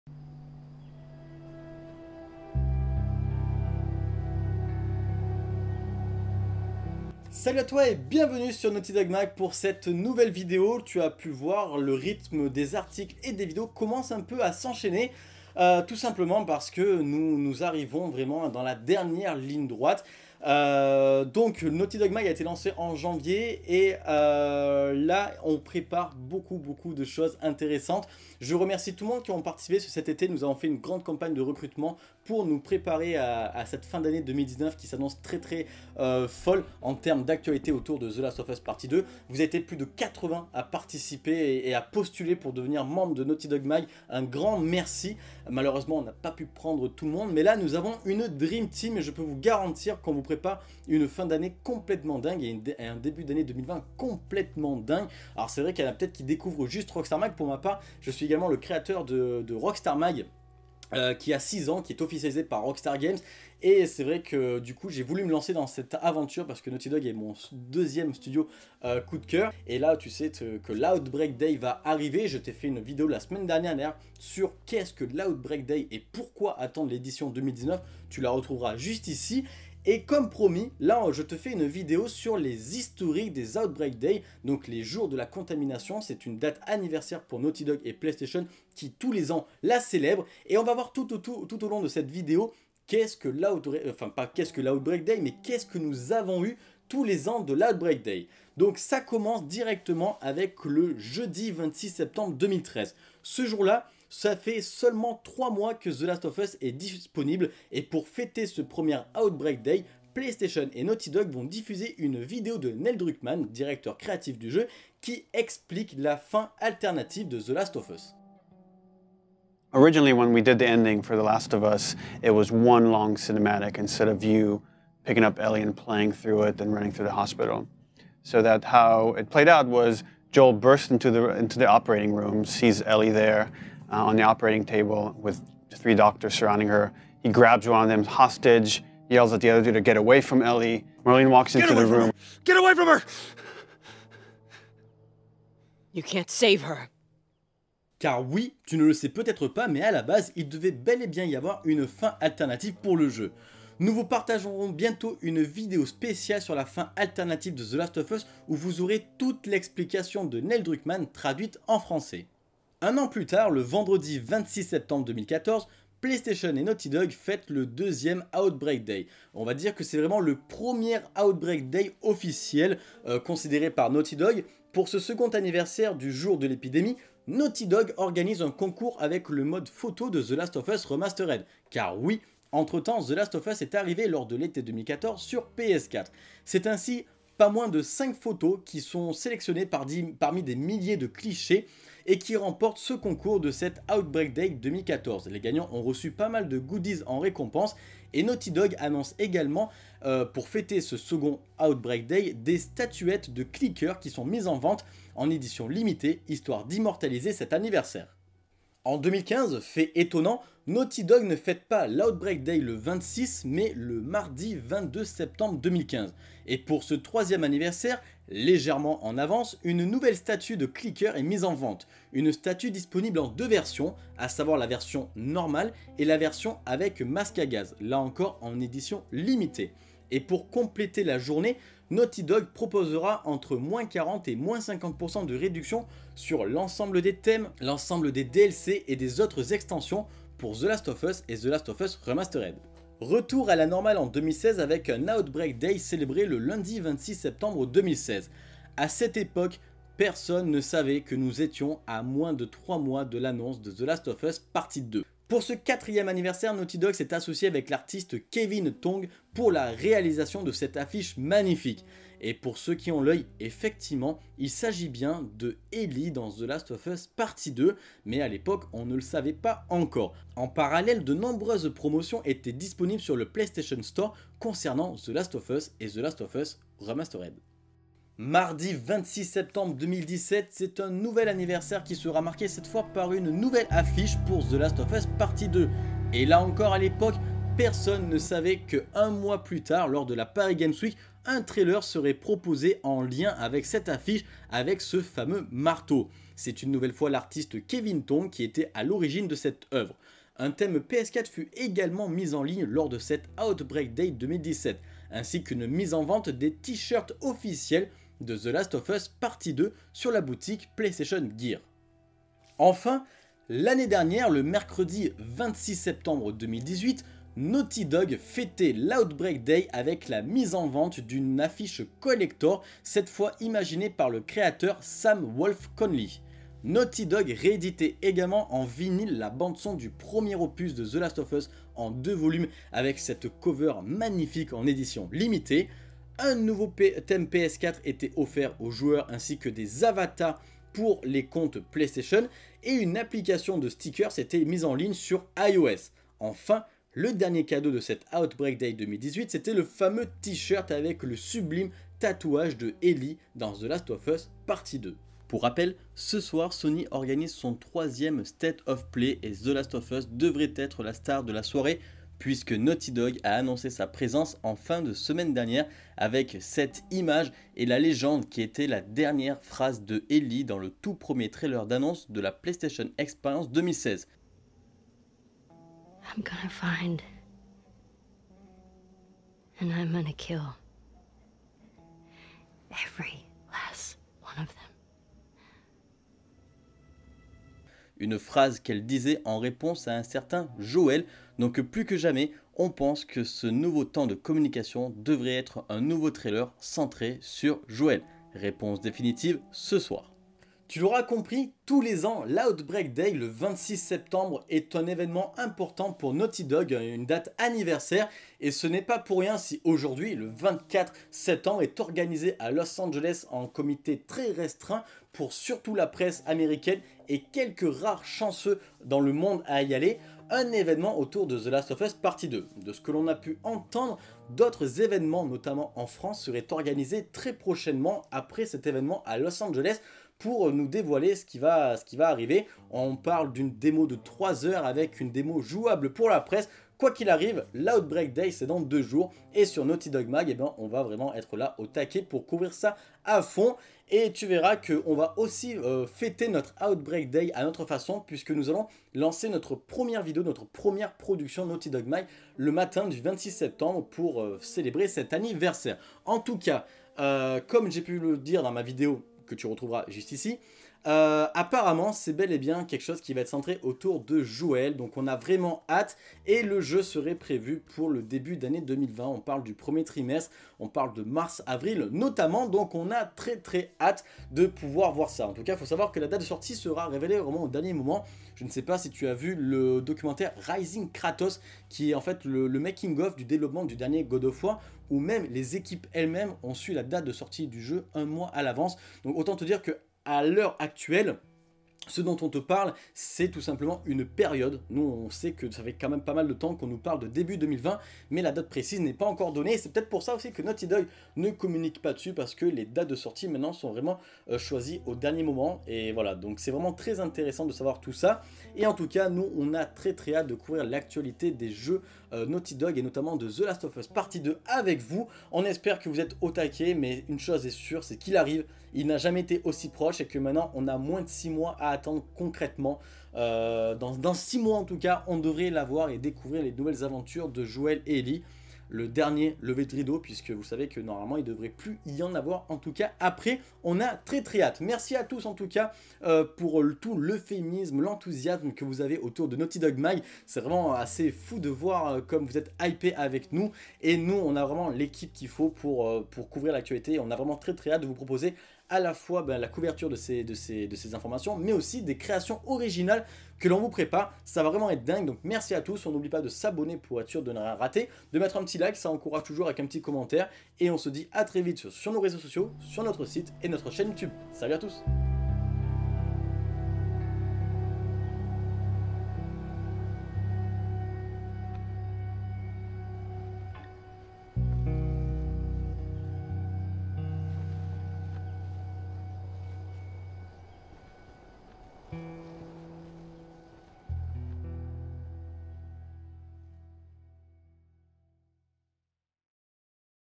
THE LAST OF US - HISTORIQUE DE TOUS LES OUTBREAK DAY (2013-2019)
La semaine dernière, on vous proposait une vidéo pour vous expliquer qu'est-ce que l'Outbreak Day et pourquoi il fallait surveiller l'édition 2019. Aujourd'hui, à seulement deux jours de cet Outbreak Day 2019, nous vous proposons un petit historique de tous les Outbreak Day !